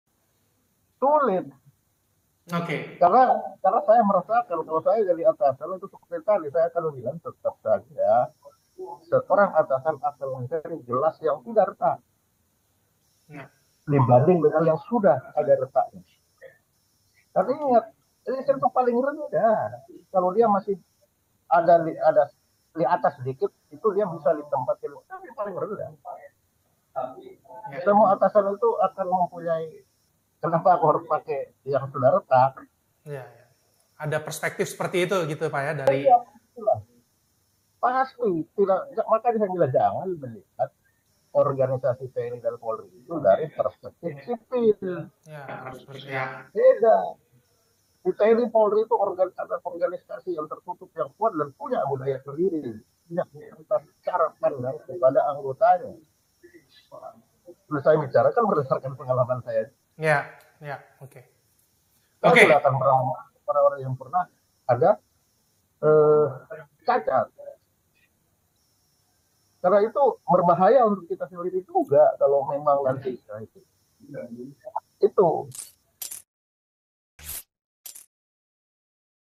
RICHARD ELIEZER KEMBALI KE POLRI, SISTEM PELINDUNG INTERNAL SULIT DIBUAT?
Mantan Kabais TNI, Soleman B Ponto menjelaskan bahwa akan sulit bagi Polri untuk membuat sistem yang melindungi Richard Eliezer jika bergabung kembali ke Polri. Menurut Ponto, jika ia menjadi atasan, maka ia akan mencari gelas yang tidak retak dibanding yang sudah retak.